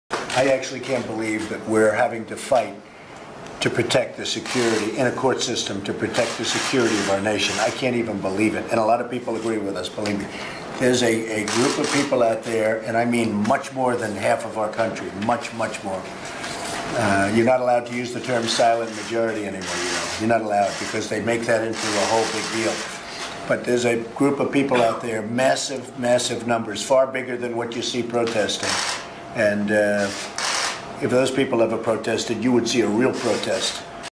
Silent Majority Fallacy - Trump example 1 - Fallacious Trump e98
The Silent Majority Fallacy is committed when someone assumes all or most of the people who aren't vocally disagreeing with them, in fact agree with them.